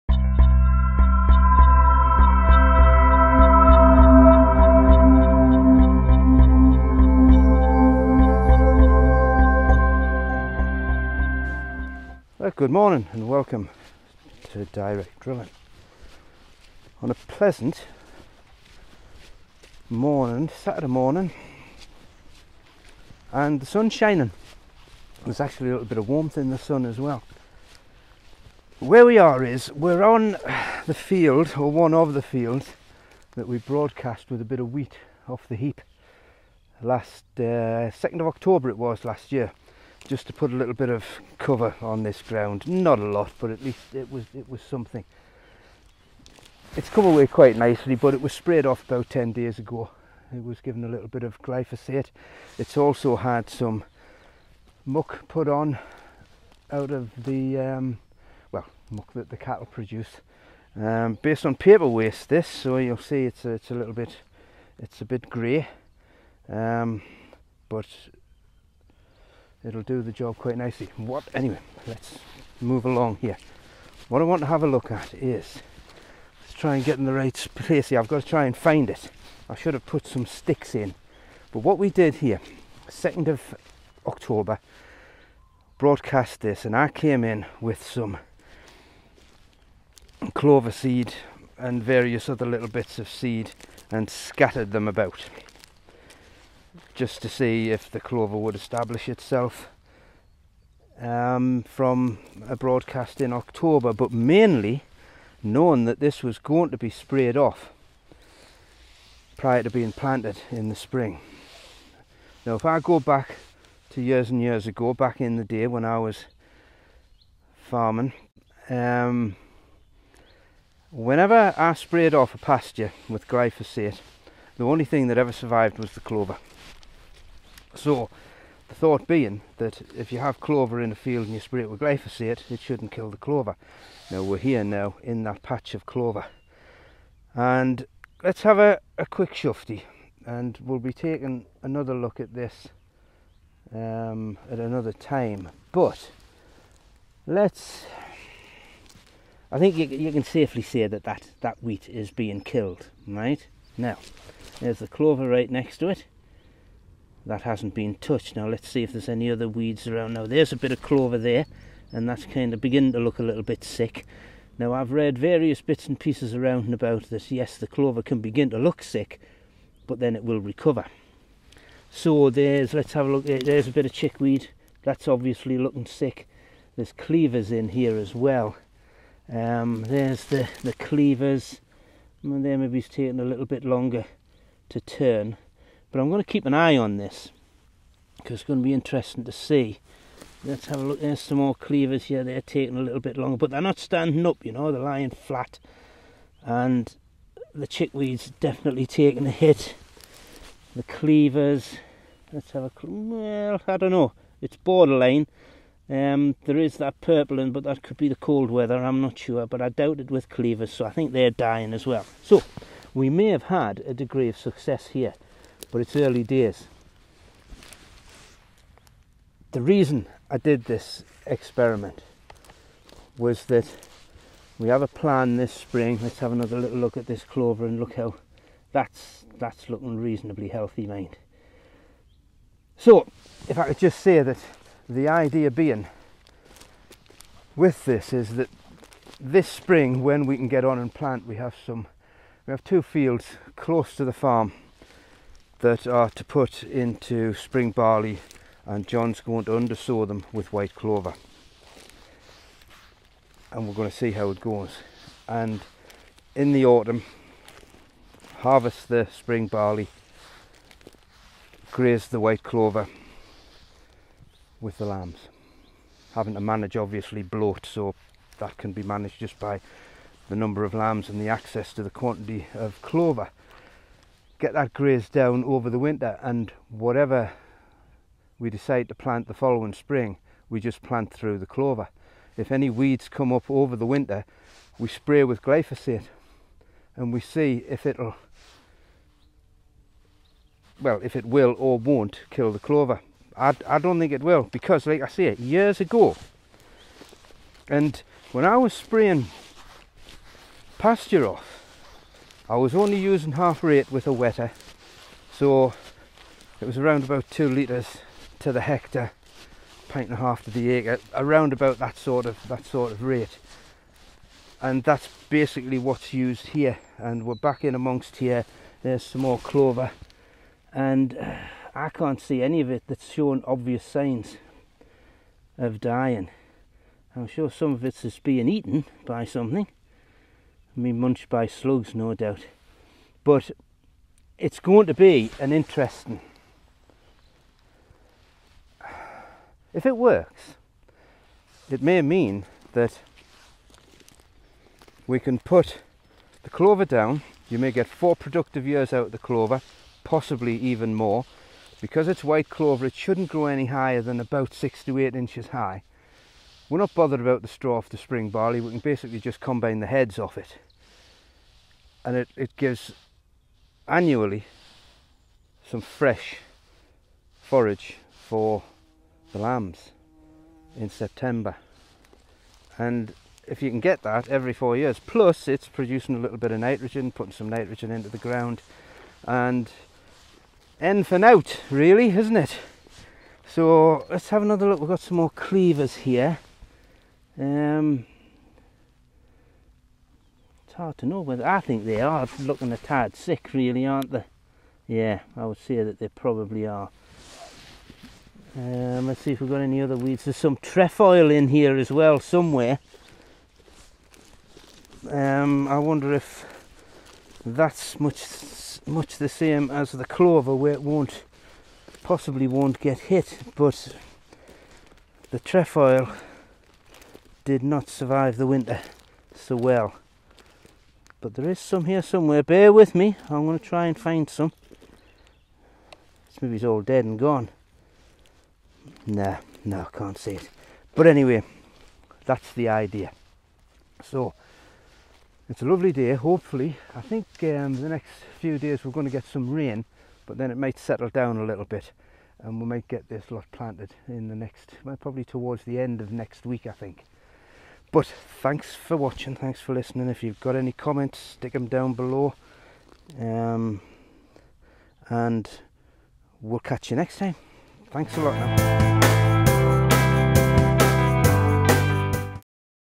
Glyphosate on Clover Report 1
This is the first report on the Clover as a permanent nutrient crop experiment to which we hope to plant successive crops through. This test is to see how the clover responds to Glyphosate as it has been known to either resist or recover from applications. This is a small hand sown area in a block that is destined for spring wheat.